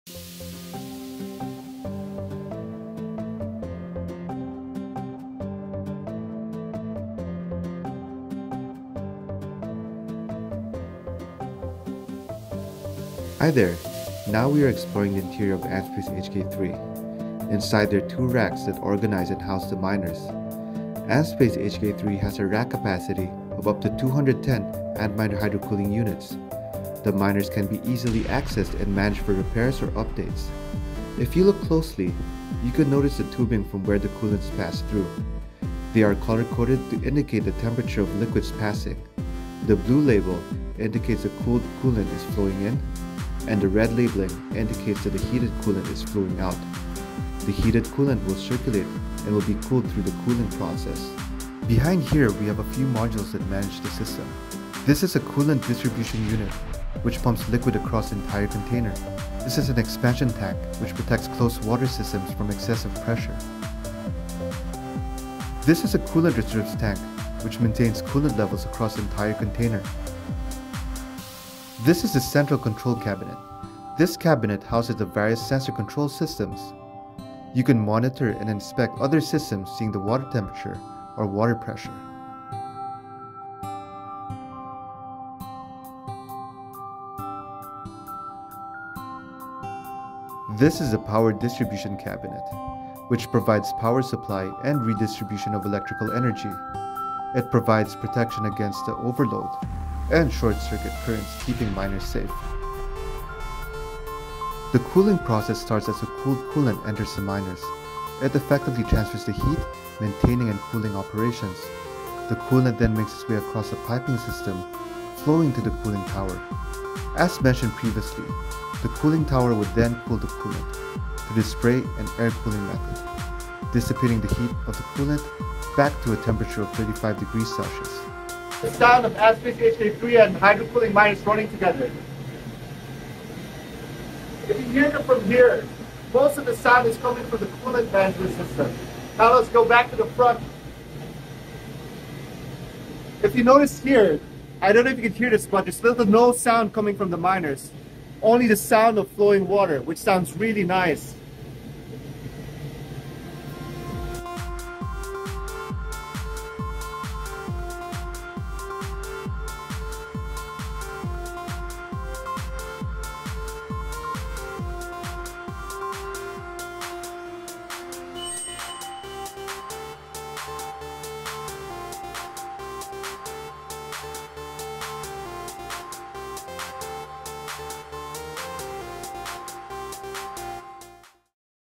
A Look Inside ANTSPACE HK3
ANTSPACE HK3 is the latest ANTMINER hydro container developed by BITMAIN, which integrates hydro cooling technologies into mining operations. It houses up to 210 ANTMINER hydro cooling units and can operate during any season of the year.